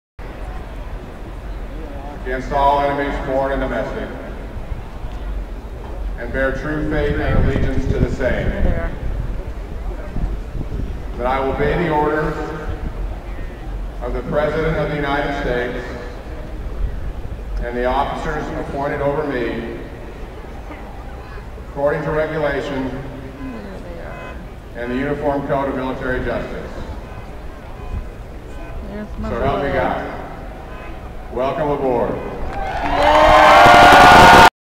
Armed Forces Bowl 2009 Recruit Swear-In 018.avi
Jake being sworn in to the USN by the Chairman of the Joint Chiefs of Staff Adm Mullen.  This was held during half-time of the Armed Forces Bowl game in Ft. Worth on 12/31/09.